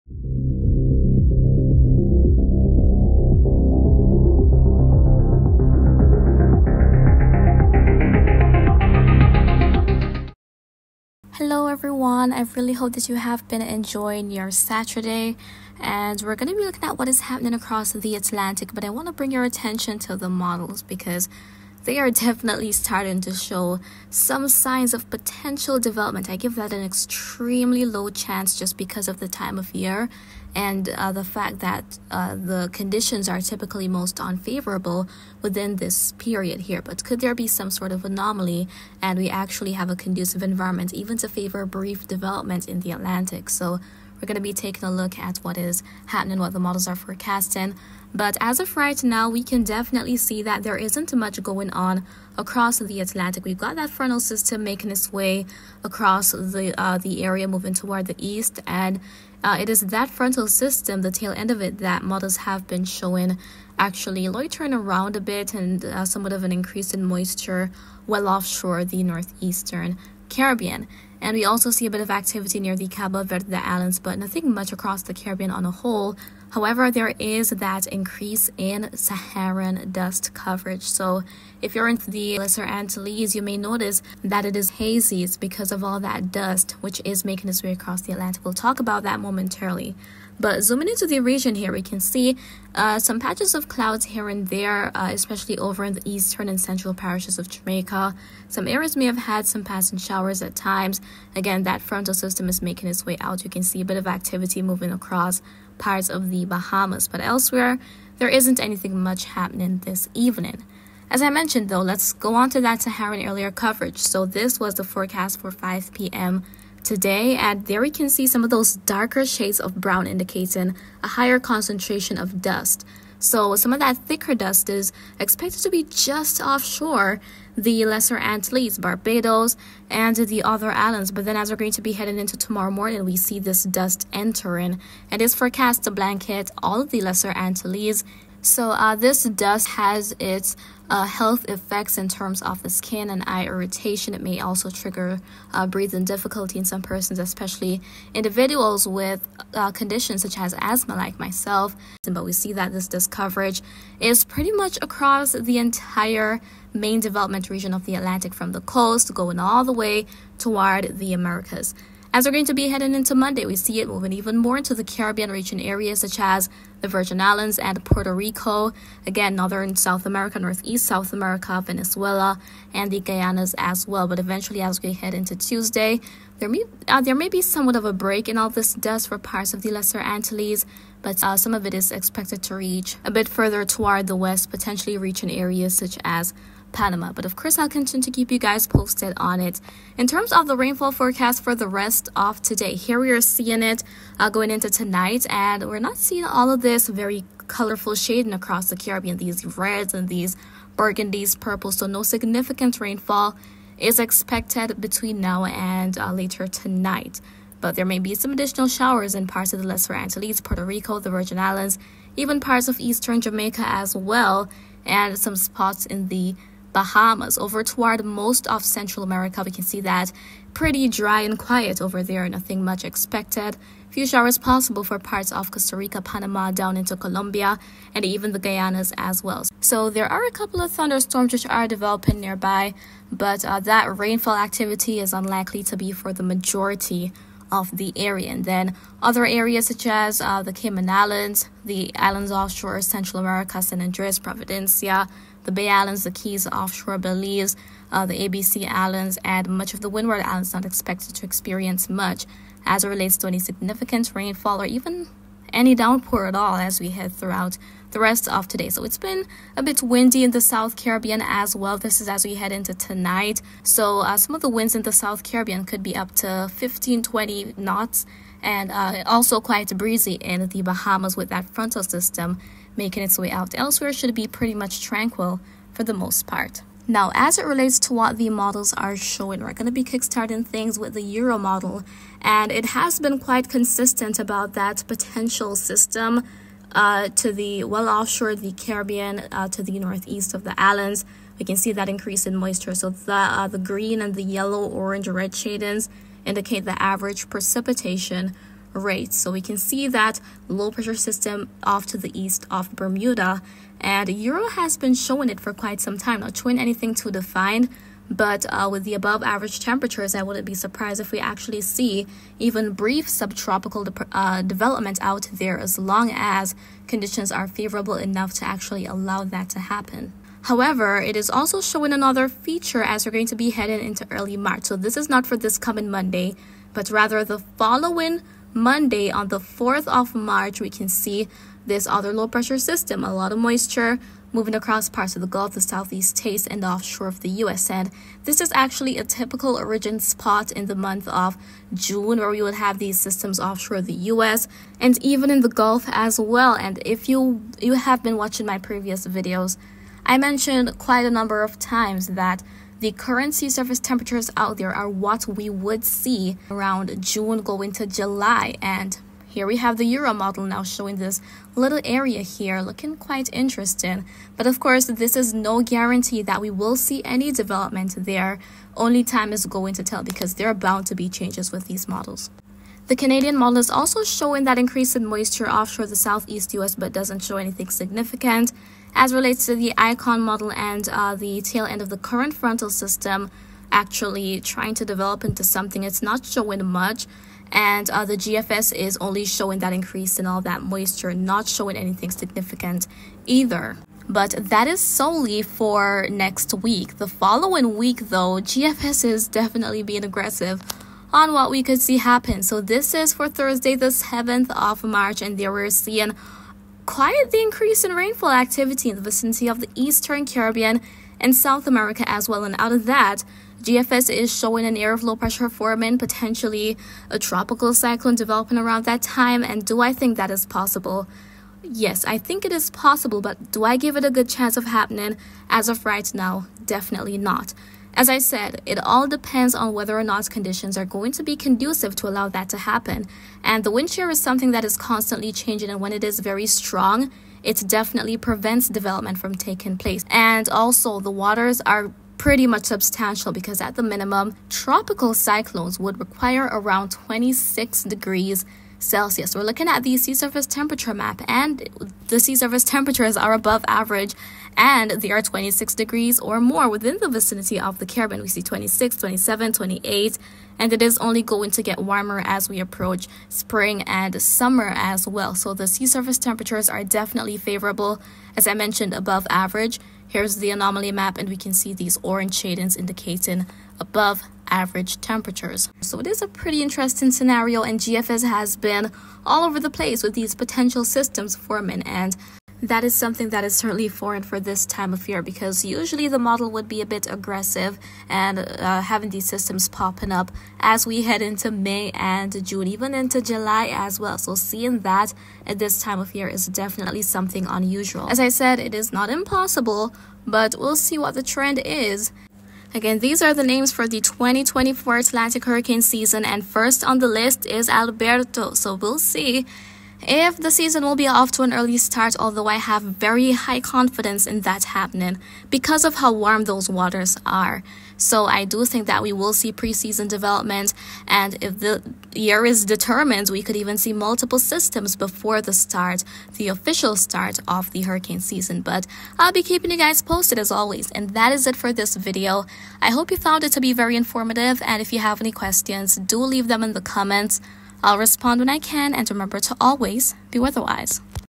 Low Pressure System(s) Forecast by Models, Caribbean Weather Update • 24/02/24
It's not the typical time of year to be discussing the possible of seeing tropical or even subtropical development but models are certainly hinting at a couple low pressure areas that may try to take advantage of the anomalously warm waters, though not guaranteed to happen.